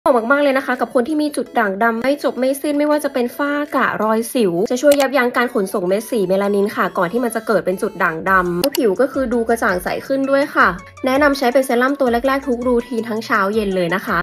จบทุกปัญหาการเกิดซ้ำของจุดด่างดำแบบวนลูป​​
อยากจบทุกปัญหาการเกิดซ้ำจุดด่างดำแบบวนลูป​

ลองนี่ Glycolic-Bright Anti-Dark Spot Brightening Serum จากลอรีอัล ปารีส ช่วยยับยั้งการขนส่งเมลานินที่เป็นต้นเหตุของจุดด่างดำ เพื่อผิวกระจ่างใสใช้เลย​

ดูข้อมูลผลิตภัณฑ์เพิ่มเติมได้ที่